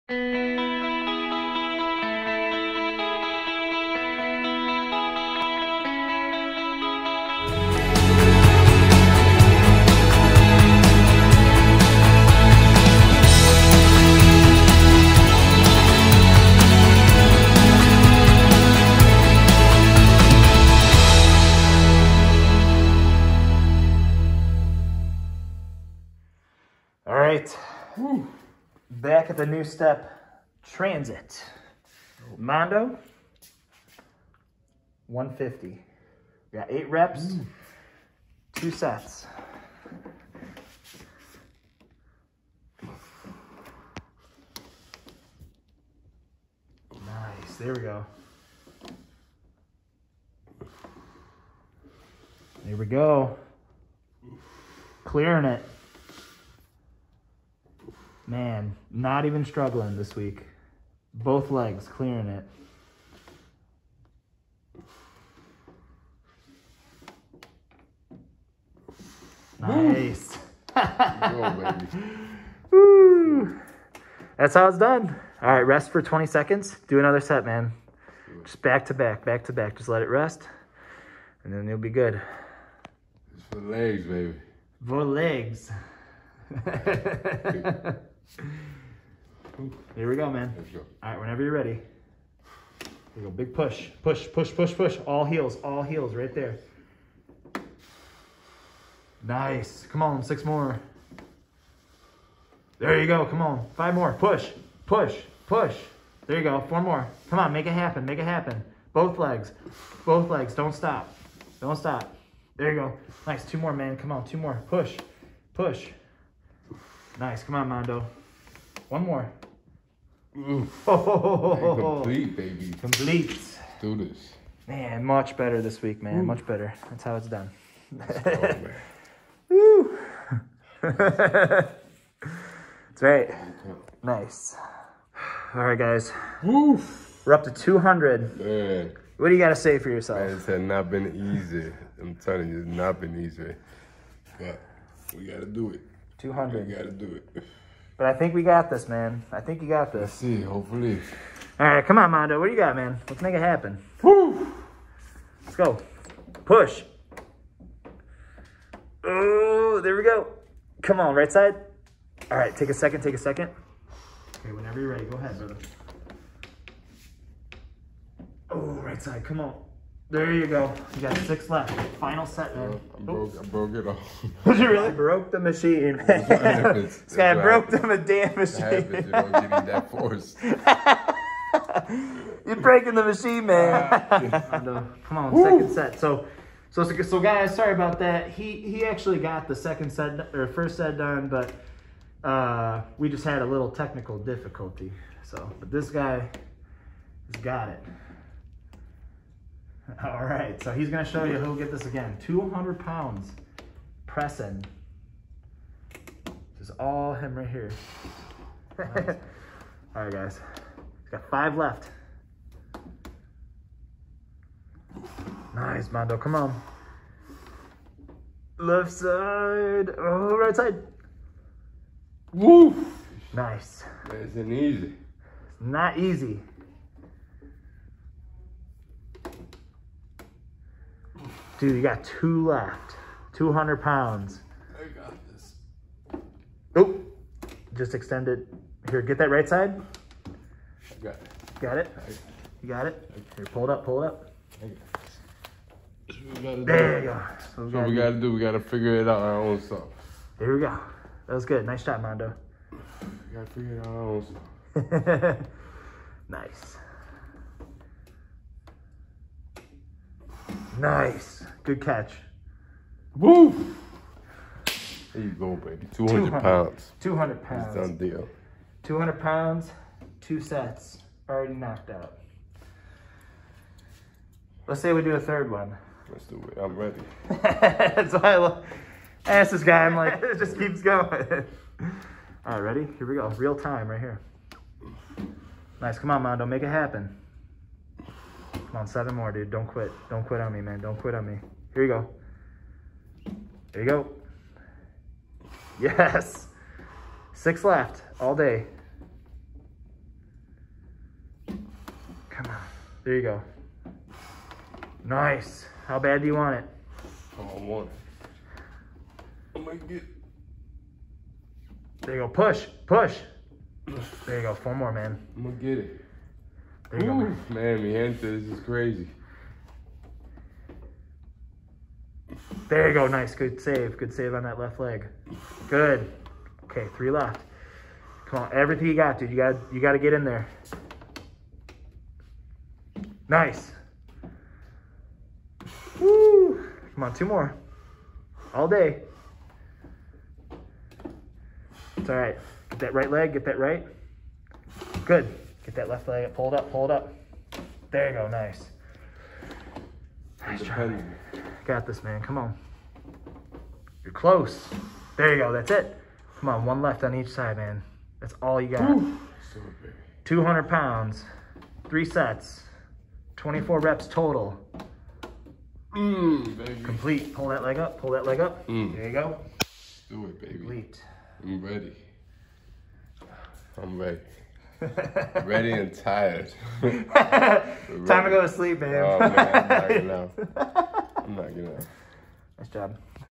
Living With MS: Thursday PT’s/ Leg Pressing
Towards the last part of my session (when pressing 200) my left leg kept hyperextending towards the back, so it would lock back, and that’s when i to manually pull my leg up and place back in correct position. It hurt...but nothing stopping ur boi!!! 😉👍🏼💪🏼